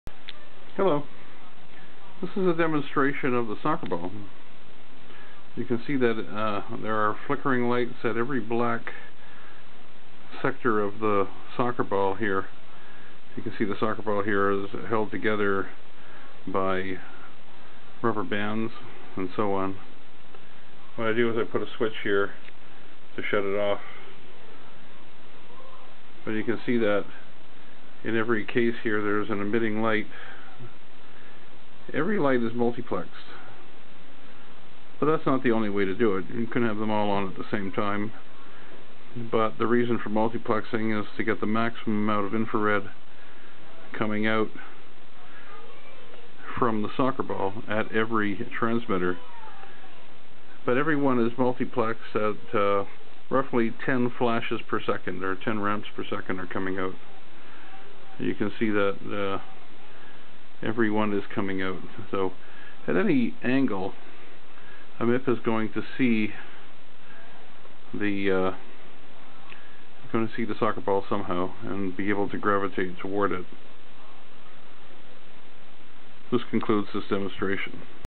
infrared Soccer ball
This is a toy soccer ball with IR emitters attached at every black patch of the soccer ball. The soccer ball is meant to be sensed by MIP robots that will gravitate toward the ball, as if in a simulated swarming robot soccer game.